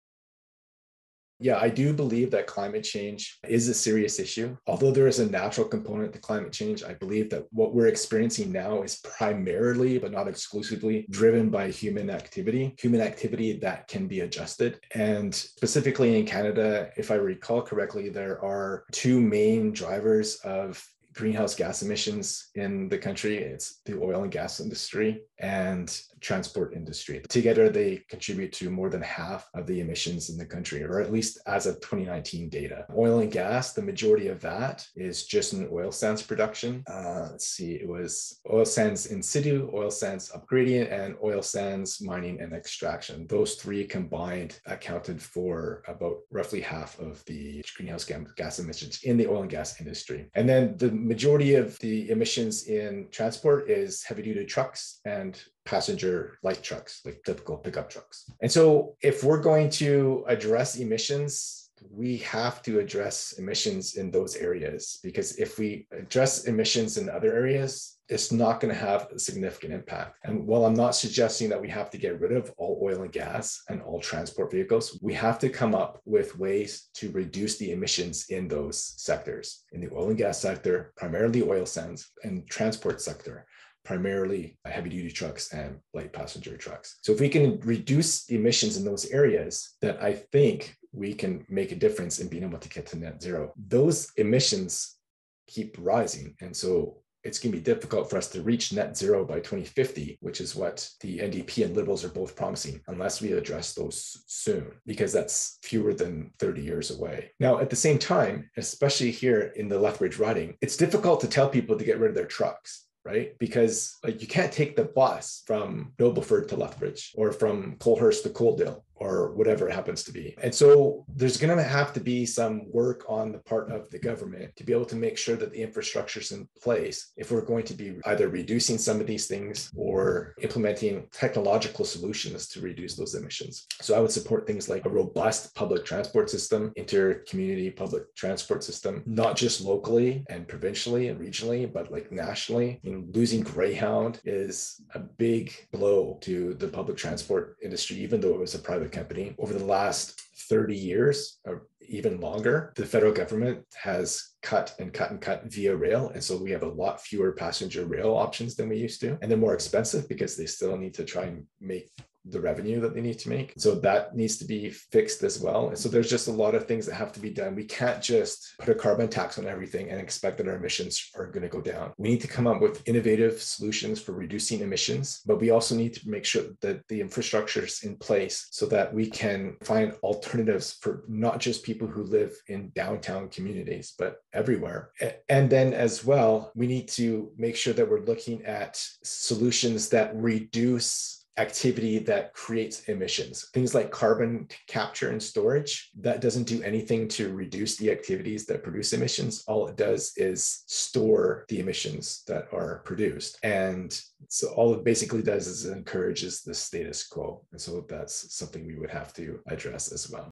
Election 2021: My thoughts on climate change
Another clip from last week’s fireside chat. My thoughts on climate change.